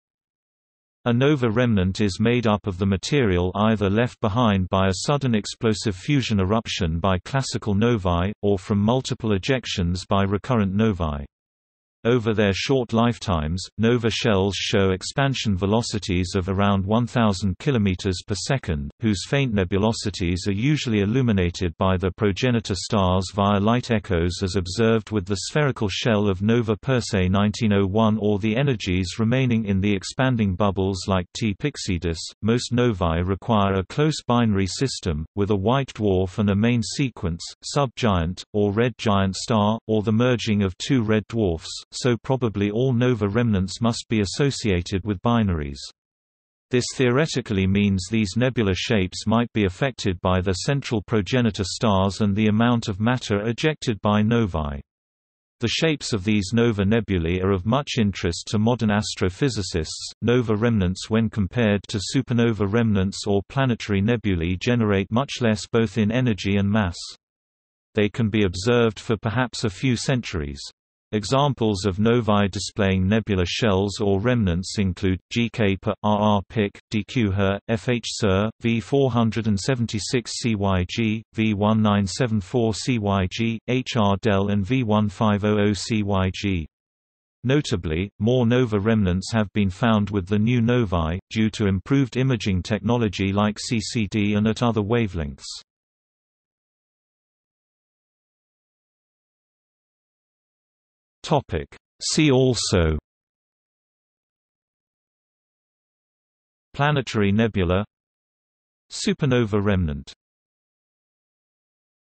Nova remnant | Wikipedia audio article
00:01:37 1 See also

- Socrates

SUMMARY
A nova remnant is made up of the material either left behind by a sudden explosive fusion eruption by classical novae, or from multiple ejections by recurrent novae. Over their short lifetimes, nova shells show expansion velocities of  around 1000 km/s, whose faint nebulosities are usually illuminated by their progenitor stars via light echos as observed with the spherical shell of Nova Persei 1901 or the energies remaining in the expanding bubbles like T Pyxidis.Most novae require a close binary system, with a white dwarf and a main sequence, sub-giant, or red giant star, or the merging of two red dwarfs, so probably all nova remnants must be associated with binaries. This theoretically means these nebula shapes might be affected by their central progenitor stars and the amount of matter ejected by novae. The shapes of these nova nebulae are of much interest to modern astrophysicists.Nova remnants when compared to supernova remnants or planetary nebulae generate much less both in energy and mass. They can be observed for perhaps a few centuries. Examples of novae displaying nebula shells or remnants include: GK Per, RR Pic, DQ Her, FH Ser, V476 Cyg, V1974 Cyg, HR Del and V1500 Cyg. Notably, more nova remnants have been found with the new novae, due to improved imaging technology like CCD and at other wavelengths.